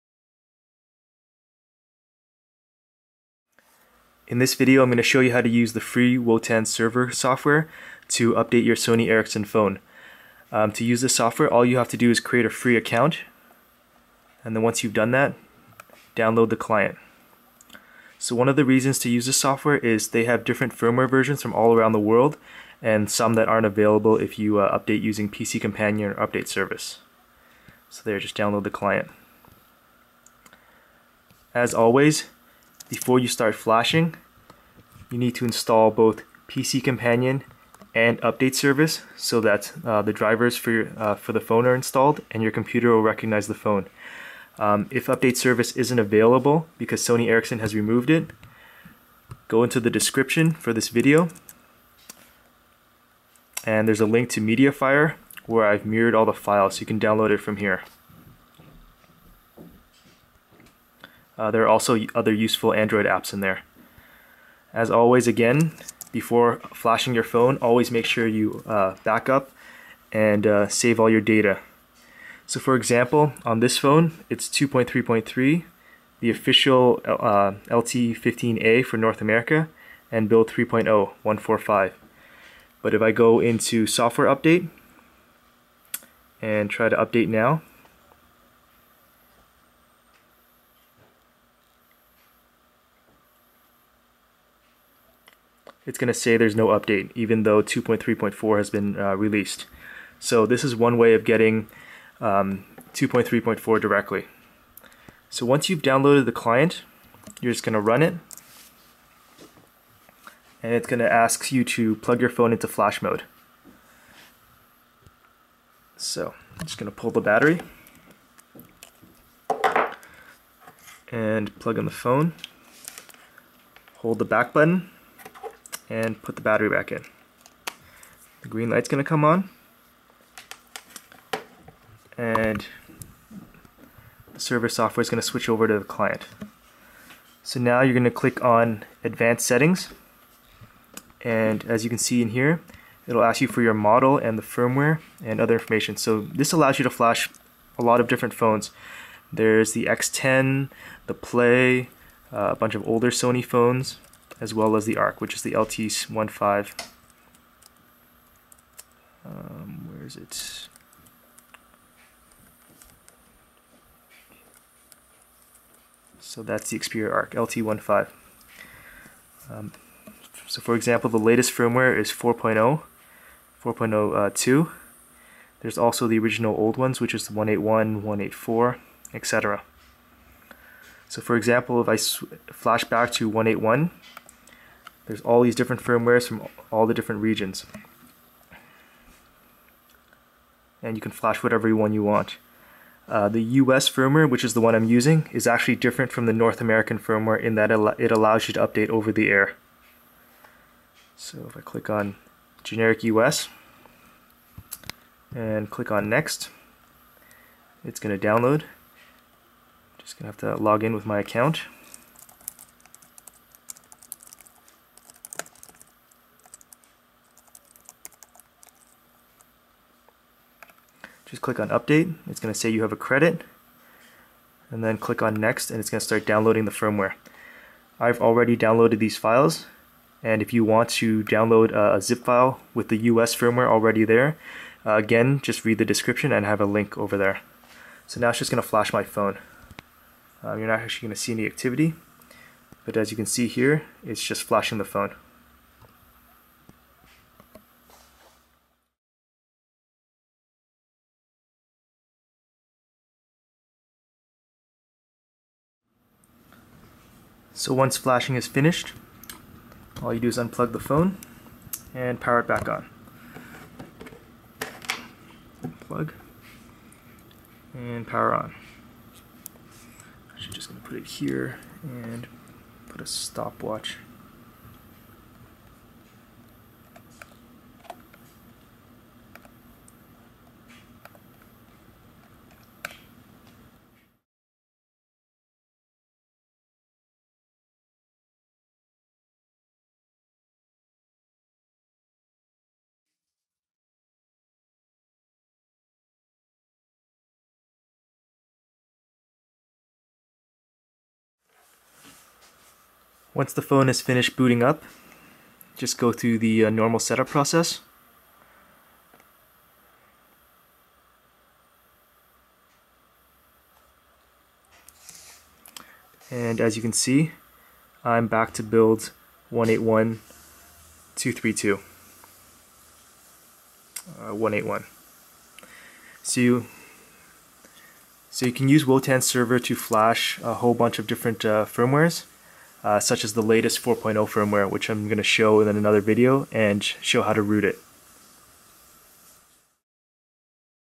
Flashing Sony Ericsson phones (arc, X10, Play, Neo, etc.) using the PAID Wotan Client
UPDATE: 2012-04: Wotan is no longer free. Use the FlashTool.

In this video, I show how to use the paid Wotan Client to flash your Sony Ericsson phone. While I'm only flashing my arc, it works on the X10, Play, Neo and many others.

to see the list of supported phones, create a free account and download the client (located under

Wotan Server has many different firmwares from all around the world for many different build versions.e.g. v2.3.2, v.2.3.3, v2.3.4, Build 4.0.A.2.368, etc.

The NEWEST Xperia firmwares as of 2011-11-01:
arc: 4.0.2.A.042
X10: 3.0.1.G.0.75

Typically, it is the firmware with the higher number, obviously.

Your phone does NOT have to be rooted (flashing wipes EVERYTHING annyways).
Your bootloader does NOT have to be unlocked (it also does NOT unlock your bootloader).
It does NOT SIM/network unlock your phone. It does NOT re-lock your phone if your phone is already unlocked. SIM/network unlocking is completely independent of any firmware and in unaffected by any flashing.

You can flash ANY corresponding firmware onto ANY corresponding phone regardless of SIM lock, bootloader condition or root.
e.g. flash a generic PLAY firmware onto a branded PLAY phone and vice versa.
flash a branded Neo firmware onto a generic Neo phone.
etc.

WARNING: DO NOT downgrade from v2.3.4 back to v2.3.3 or v2.3.2 or you may experience the 0% battery issue. XDA threads with help regarding the 0% battery issue

Wotan Server firmware files are NOT convert-able to Bin4ry's FlashTool FTF files. (I've tried for a while with no luck and I've also asked for Bin4ry's help.)

As always, before flashing your phone, BACKUP all your data: APN/3G settings (use the free APN Backup & Restore available on the Market), SMS, contacts, etc.

You MUST have PC Companion installed or else your computer will not recognize the phone. Update Service is also recommended. If it isn't available at Sony, download from my mirror

NOTE: Flashing an "i" firmware onto an "a" phone will NOT give you 3G in an "i" zone (only 3G in an "a" zone). Flashing an "a" firmware onto an "i" phone will NOT give you 3G in an "a" zone (only 3G in an "i" zone). The radios are FIXED and not changable through firmware.

All v2.x firmwares will work on ANY phone. i.e. if you want German customization, you can flash the German arc firmware onto your North American arc. 3G will still work (in North America) once you enter in your APN settings.

If anything goes wrong, use PC Companion, Update Service, the FlashTool, or Wotan to flash the phone again. Unless you break the USB port, you cannot brick an Xperia phone.

Thanks!